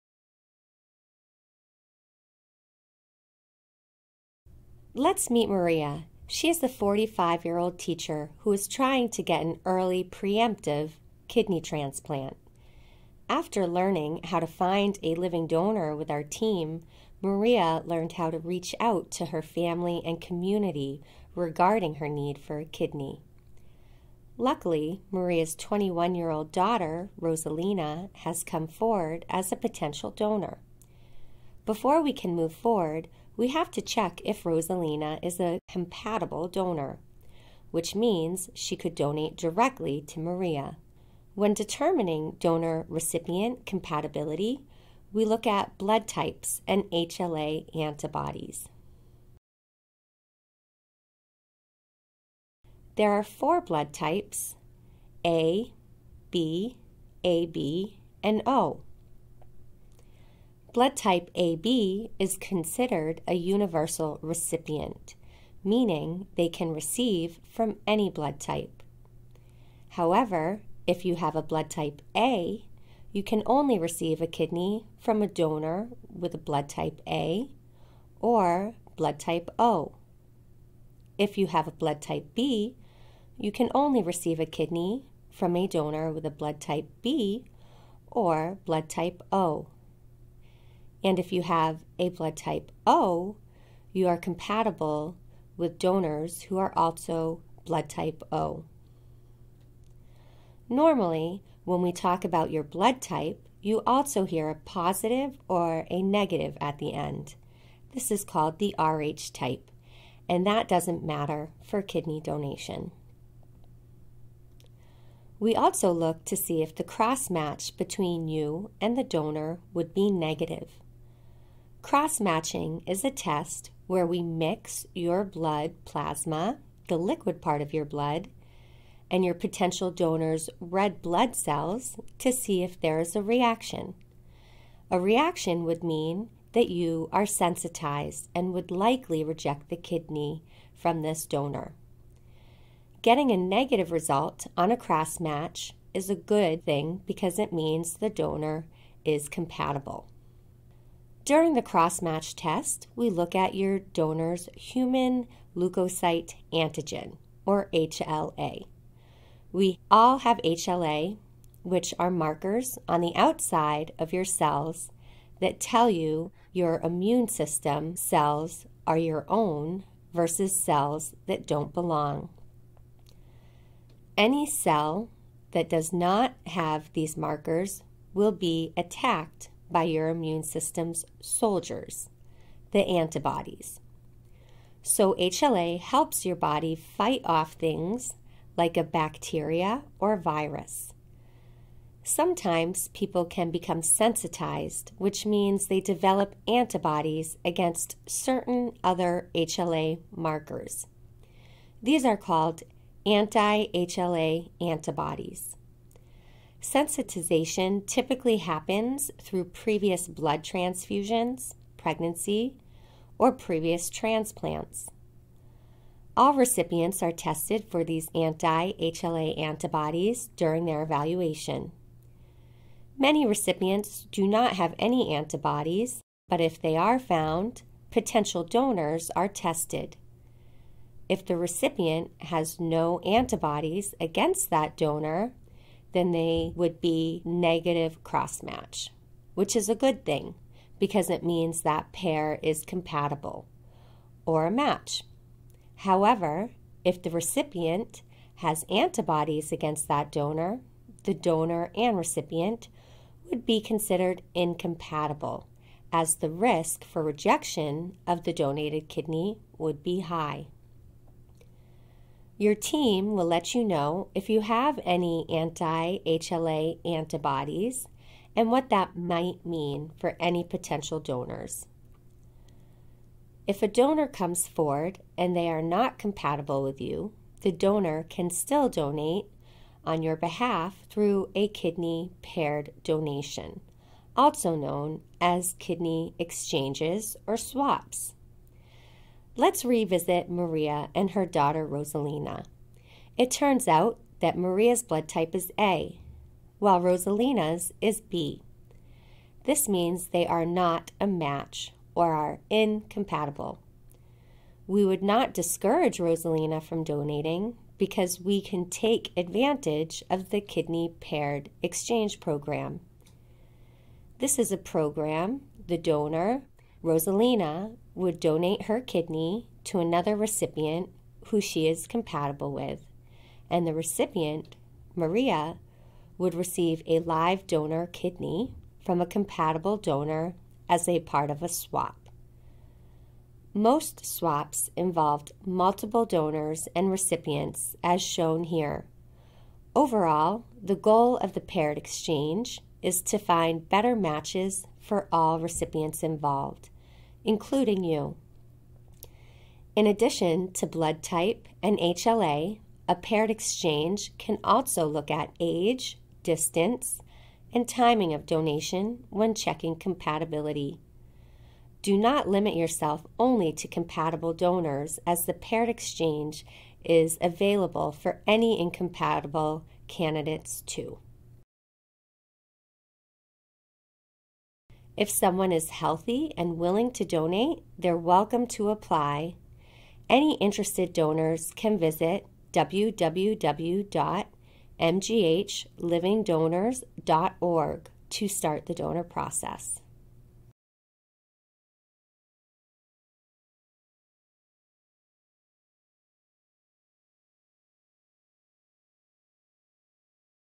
Determining Kidney Donor Compatibility
Determining Kidney Donor Compatibility: Learn about factors affecting donor-recipient compatibility and necessary testing.